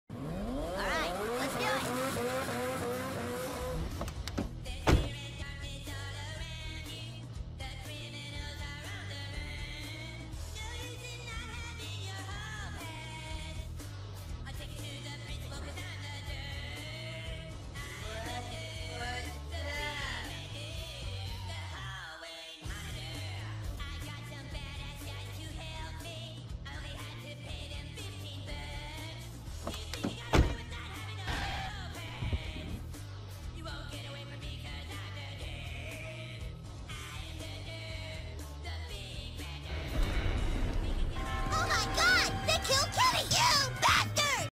Cartman gets a Ferrari
Still working out the green screen and lighting. I'll do a test next time. This is about an hour of shooting every 15 seconds.
There is another video showing how I made this which will be up soon.